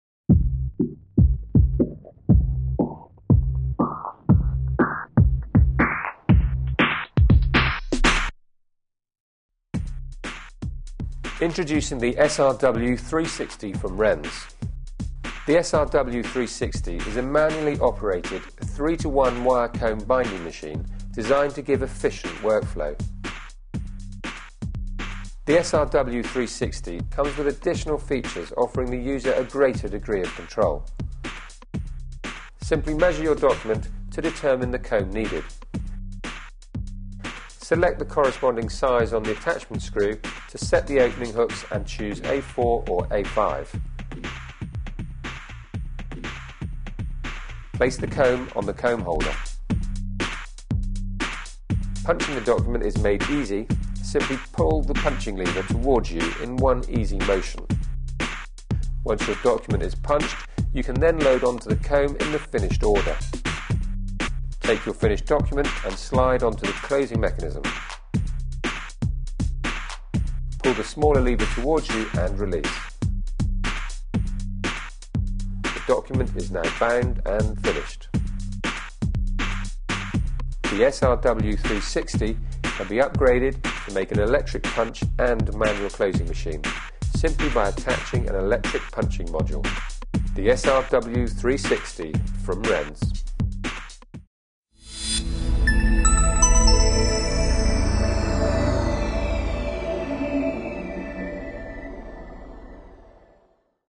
Renz SRW 360 Wire Binder
The Renz SRW 360 is a durable and rugged 3:1 pitch wire binding machine. This machine features a modern design and fits in with any office, copy shop or print room and comes with 1000 FREE No 4 binding wires. It has punching pins, all of which can be disengaged to enable odd size papers to be punched. An adjustable margin control allows you to precisely punch the paper every time.

Manual 3:1 punch square holes.
Binds up to 120 sheets.
Wire holding device.
Adjustable punching margin.
Selective punching pins.
Punches up to 25 sheets.
Metal construction.
Includes 1000 FREE A4 No 4 binding wires - available in Black, White or Silver